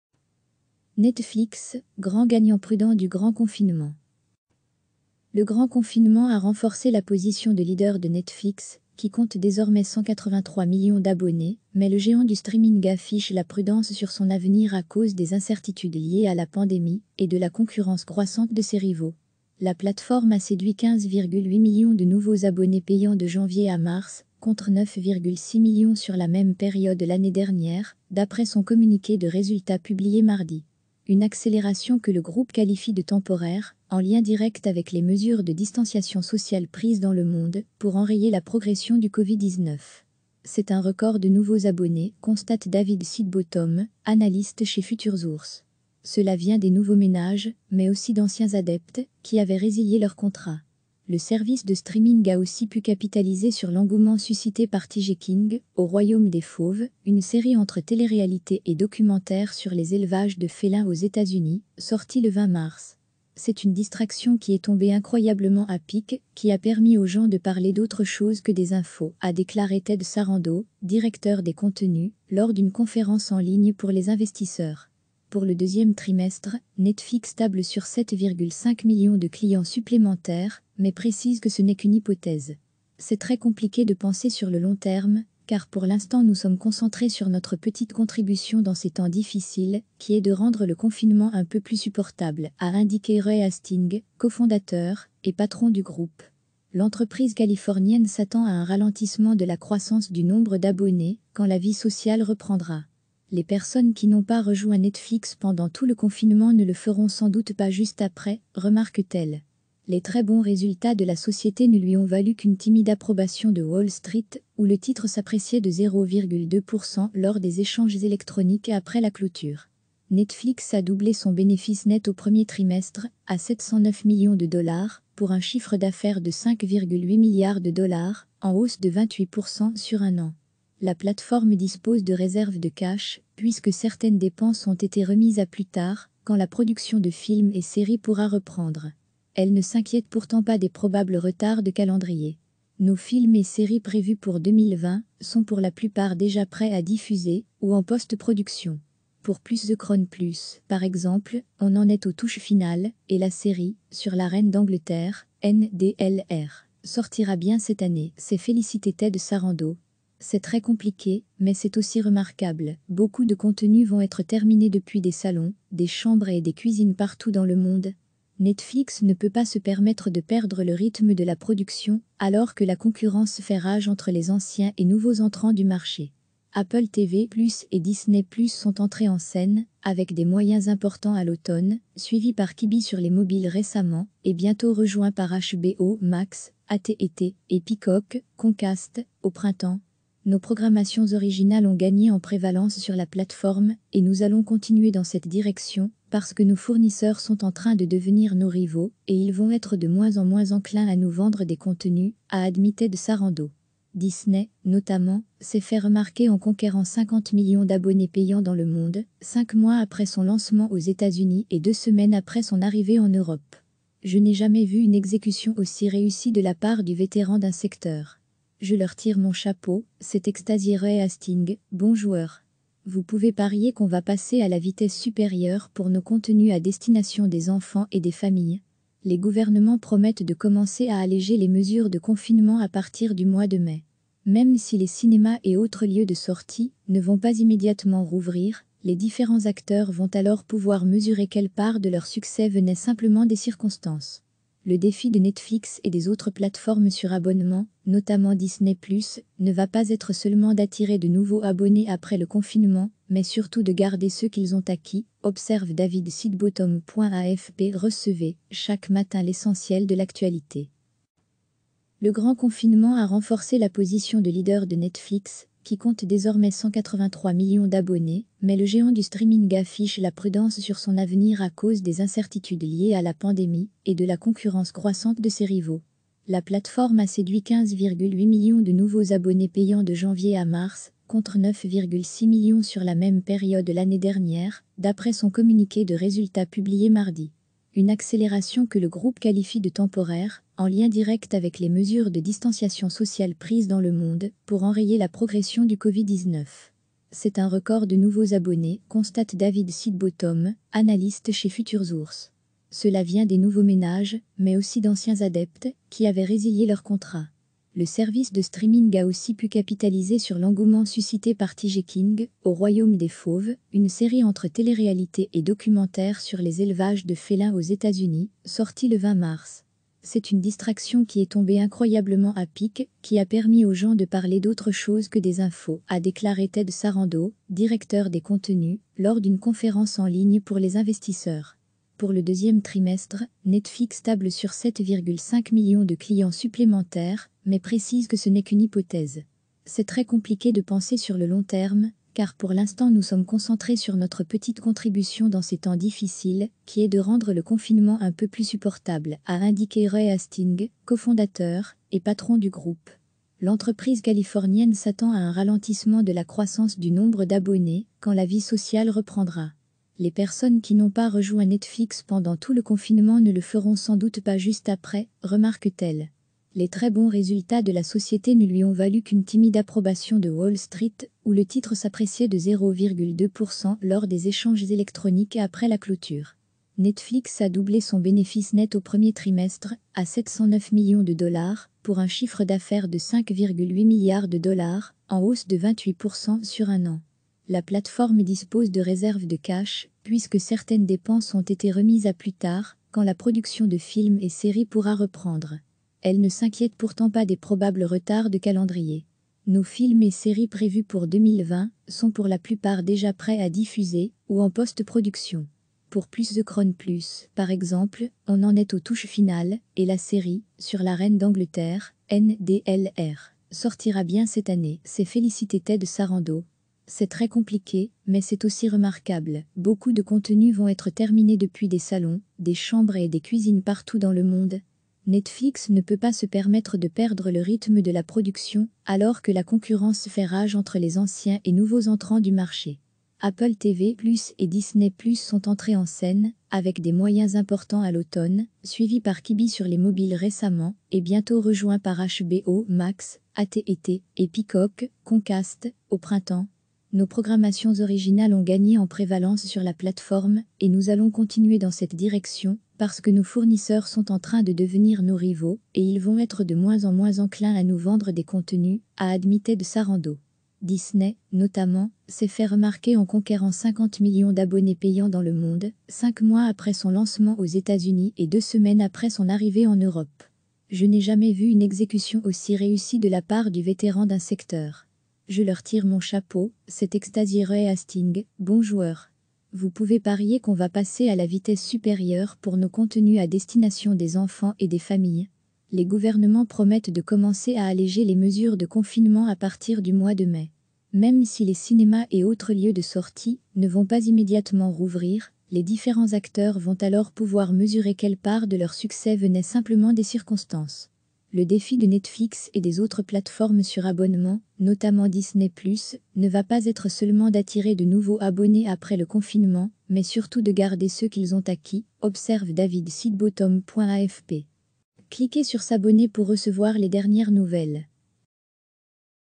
Netflix, grand gagnant prudent du "Grand confinement"
Le "Grand confinement" a renforcé la position de leader de Netflix, qui compte désormais 183 millions d’abonnés, mais le géant du streaming affiche la prudence sur son avenir à cause des incertitudes liées à la pandémie et de la concurrence croissante de ses rivaux.

La plateforme a séduit 15,8 millions de nouveaux abonnés payants de janvier à mars, contre 9,6 millions sur la même période l’année dernière, d’après son communiqué de résultats publié mardi.

Une accélération que le groupe qualifie de "temporaire", en lien direct avec les mesures de distanciation sociale prises dans le monde pour enrayer la progression du Covid-19.

"C’est un record de nouveaux abonnés", constate David Sidebottom, analyste chez Futuresource. "Cela vient des nouveaux ménages, mais aussi d’anciens adeptes qui avaient résilié leur contrat".

Le service de streaming a aussi pu capitaliser sur l’engouement suscité par "Tiger King" ("Au Royaume des fauves"), une série entre téléréalité et documentaire sur les élevages de félins aux Etats-Unis, sortie le 20 mars.

"C’est une distraction qui est tombée incroyablement à pic, qui a permis aux gens de parler d’autre chose que des infos", a déclaré Ted Sarandos, directeur des contenus, lors d’une conférence en ligne pour les investisseurs.

Pour le deuxième trimestre, Netflix table sur 7,5 millions de clients supplémentaires, mais précise que ce n’est qu’une "hypothèse".

"C’est très compliqué de penser sur le long terme, car pour l’instant nous sommes concentrés sur notre petite contribution dans ces temps difficiles, qui est de rendre le confinement un peu plus supportable", a indiqué Reed Hastings, cofondateur et patron du groupe.

L’entreprise californienne s’attend à un ralentissement de la croissance du nombre d’abonnés quand la vie sociale reprendra.

"Les personnes qui n’ont pas rejoint Netflix pendant tout le confinement ne le feront sans doute pas juste après", remarque-t-elle.

Les très bons résultats de la société ne lui ont valu qu’une timide approbation de Wall Street, où le titre s’appréciait de 0,2% lors des échanges électroniques après la clôture.

Netflix a doublé son bénéfice net au premier trimestre, à 709 millions de dollars, pour un chiffre d’affaires de 5,8 milliards de dollars, en hausse de 28% sur un an.

La plateforme dispose de réserves de cash, puisque certaines dépenses ont été remises à plus tard, quand la production de films et séries pourra reprendre.

Elle ne s’inquiète pourtant pas des probables retards de calendrier.

"Nos films et séries prévus pour 2020 sont pour la plupart déjà prêts à diffuser ou en post-production. Pour + The Crown +, par exemple, on en est aux touches finales, et la série (sur la reine d’Angleterre, ndlr) sortira bien cette année", s’est félicité Ted Sarandos. "C’est très compliqué, mais c’est aussi remarquable : beaucoup de contenus vont être terminés depuis des salons, des chambres et des cuisines partout dans le monde !"

Netflix ne peut pas se permettre de perdre le rythme de la production, alors que la concurrence fait rage entre les anciens et nouveaux entrants du marché.

Apple TV + et Disney + sont entrés en scène avec des moyens importants à l’automne, suivis par Quibi sur les mobiles récemment, et bientôt rejoints par HBO Max (AT&T) et Peacock (Comcast) au printemps.

"Nos programmations originales ont gagné en prévalence sur la plateforme et nous allons continuer dans cette direction parce que nos fournisseurs sont en train de devenir nos rivaux et ils vont être de moins en moins enclins à nous vendre des contenus", a admis Ted Sarandos.

Disney, notamment, s’est fait remarquer en conquérant 50 millions d’abonnés payants dans le monde, cinq mois après son lancement aux Etats-Unis et deux semaines après son arrivée en Europe.

"Je n’ai jamais vu une exécution aussi réussie de la part du vétéran d’un secteur. Je leur tire mon chapeau", s’est extasié Reed Hastings, bon joueur. "Vous pouvez parier qu’on va passer à la vitesse supérieure pour nos contenus à destination des enfants et des familles !".

Les gouvernements promettent de commencer à alléger les mesures de confinement à partir du mois de mai.

Même si les cinémas et autres lieux de sortie ne vont pas immédiatement rouvrir, les différents acteurs vont alors pouvoir mesurer quelle part de leur succès venait simplement des circonstances.

"Le défi de Netflix et des autres plateformes sur abonnement, notamment Disney +, ne va pas être seulement d’attirer de nouveaux abonnés après le confinement, mais surtout de garder ceux qu’ils ont acquis", observe David Sidebottom.AFP

Le "Grand confinement" a renforcé la position de leader de Netflix, qui compte désormais 183 millions d’abonnés, mais le géant du streaming affiche la prudence sur son avenir à cause des incertitudes liées à la pandémie e